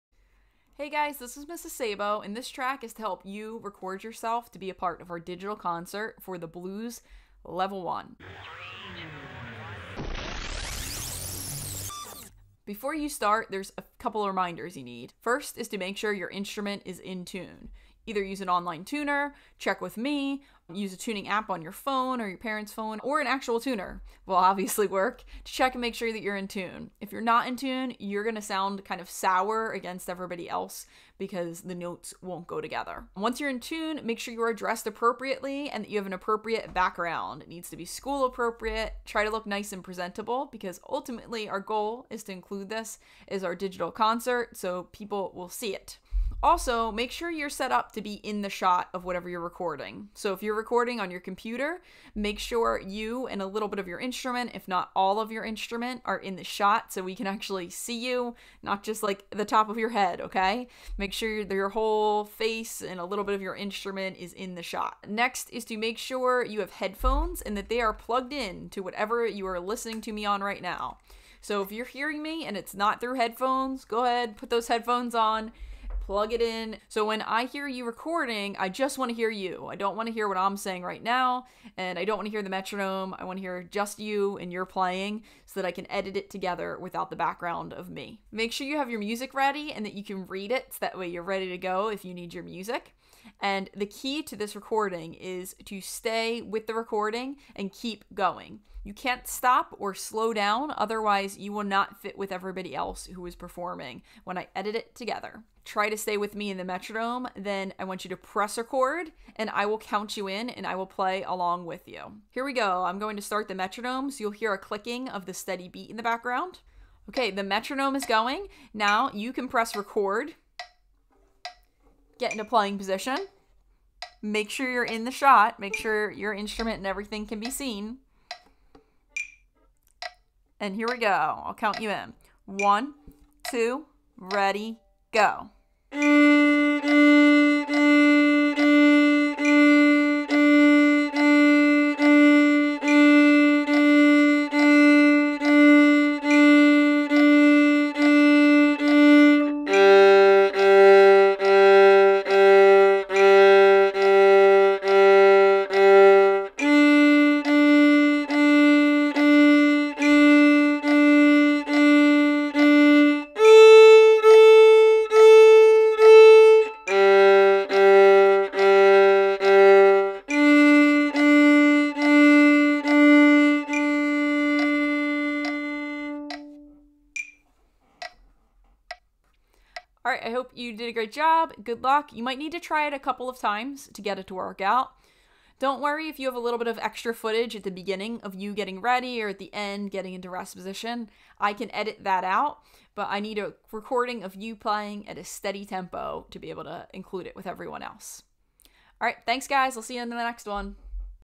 How to Record the Blues Level 1
This video will talk you through how to record yourself playing the level 1 part of The Blues while playing along with the recording.